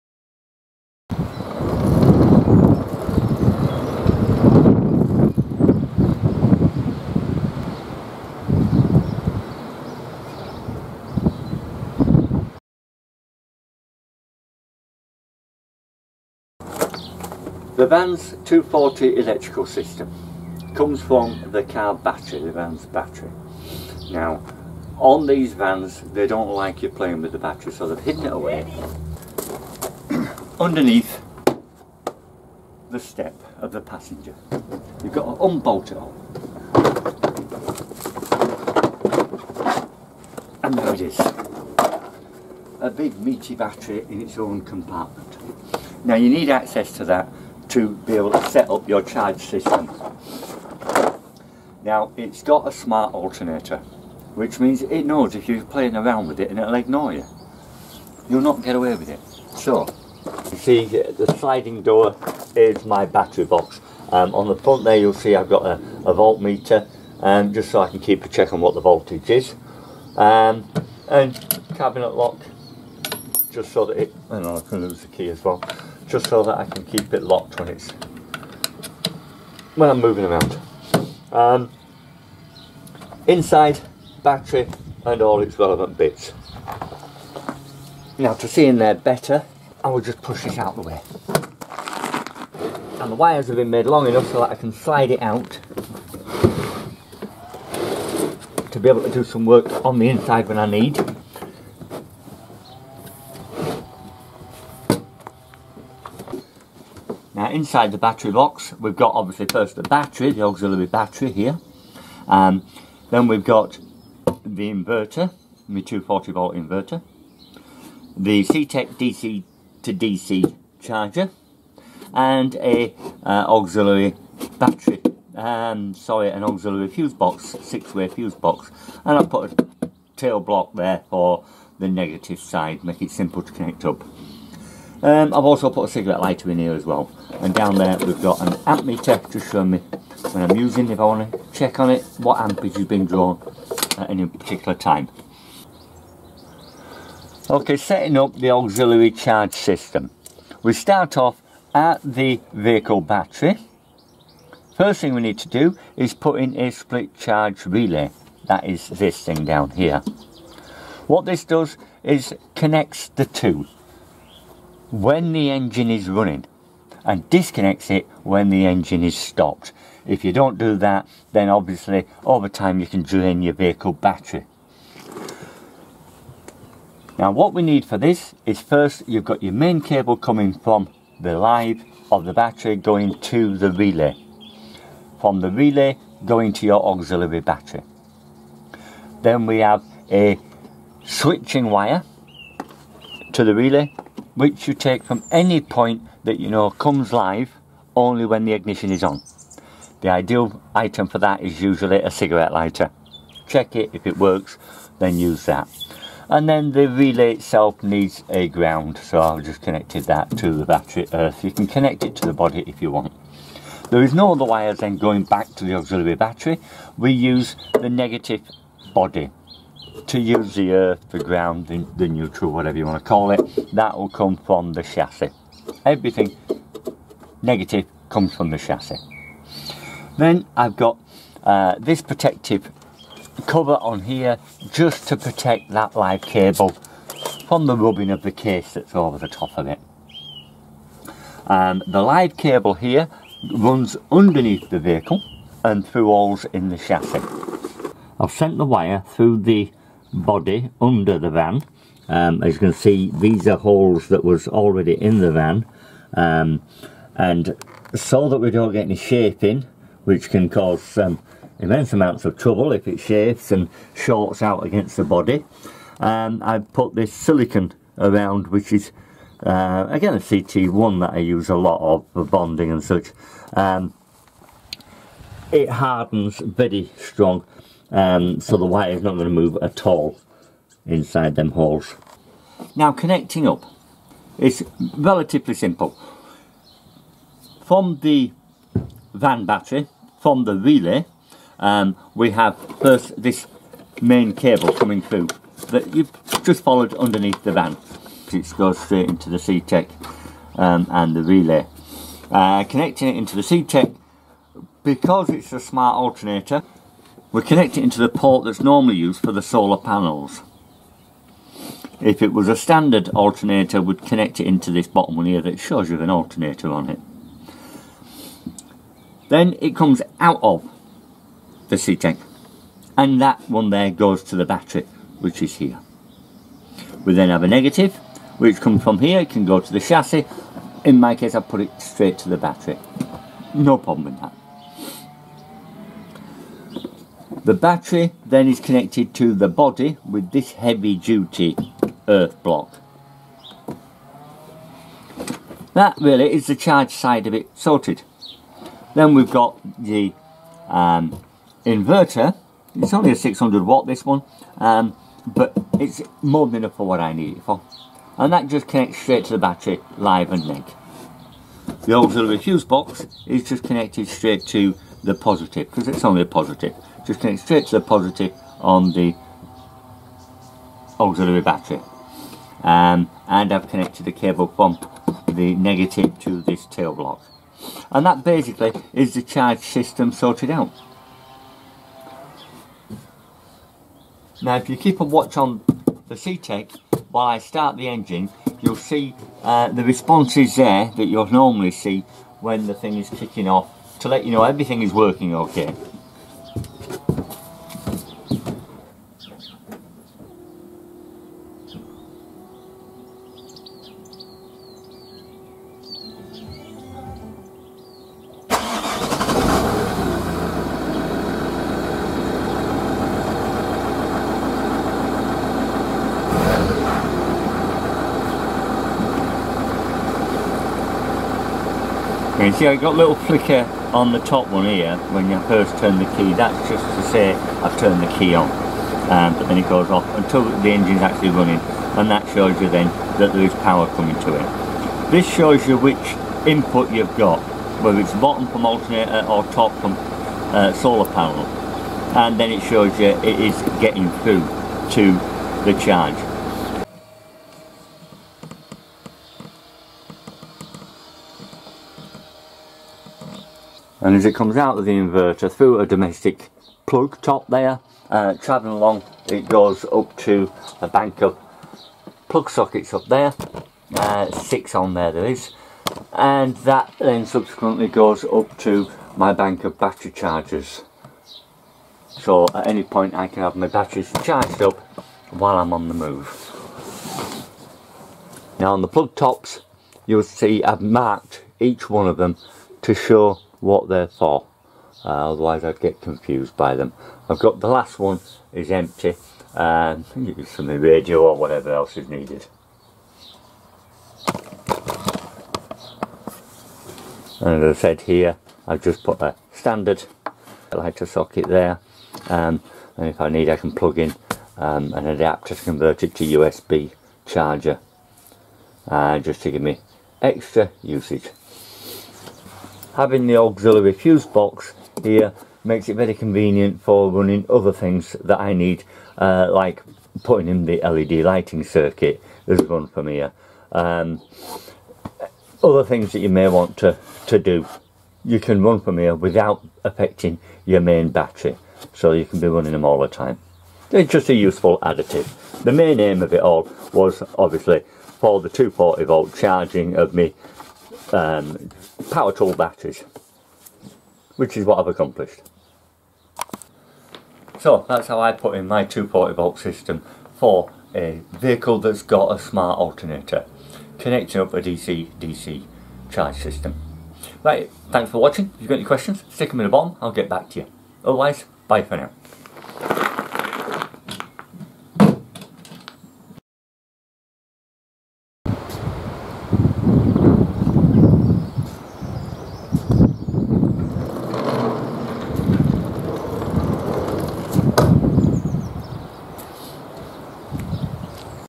Work Van DC DC Charge System
have a mains 230v supply in the van is what I have always needed and now I have it and it was quite easy to do, come have a look how easy